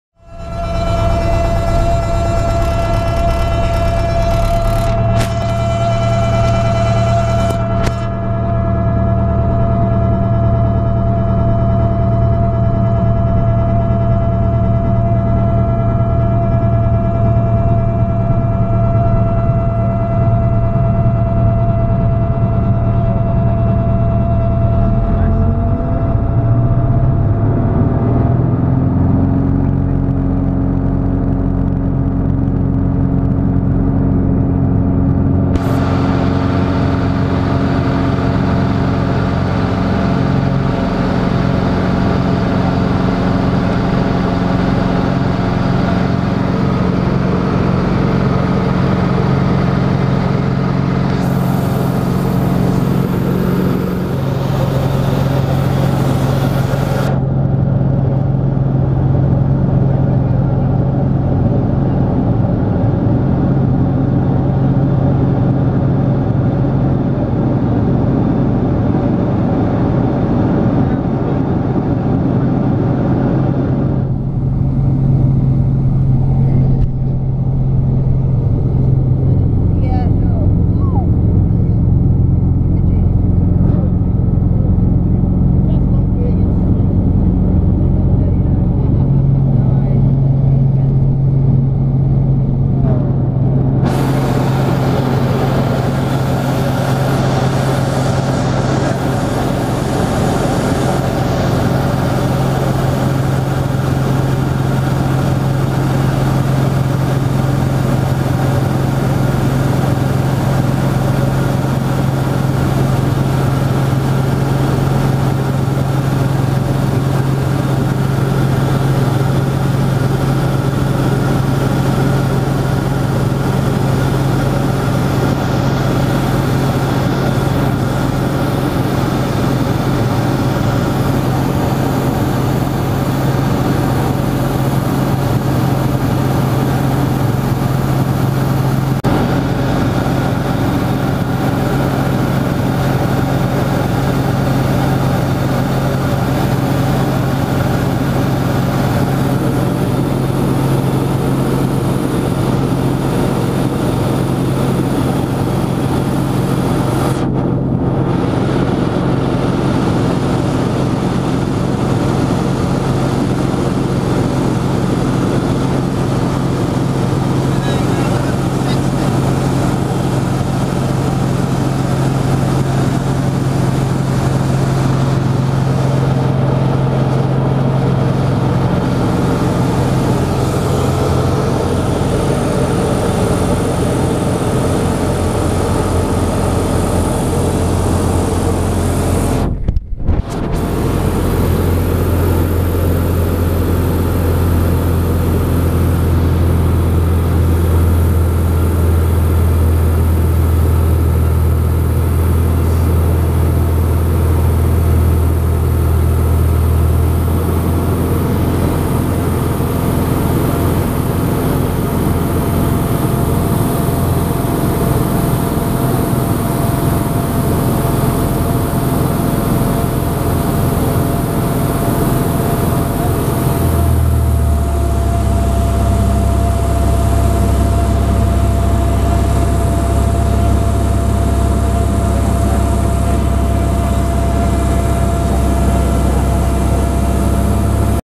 Grob G115E Tutor | Air Experience Flight (AEF) | GoPro
Join me as I take to the skies in the Grob G115E Tutor as I take on my first aerobatics flight. Through out the flight I was able to pull 5gs and 0gs. A very exhilarating experience in which I would not refuse to do again! I will never forget that moment when I 'took control'. Sit back, HOLD ON and enjoy the ride!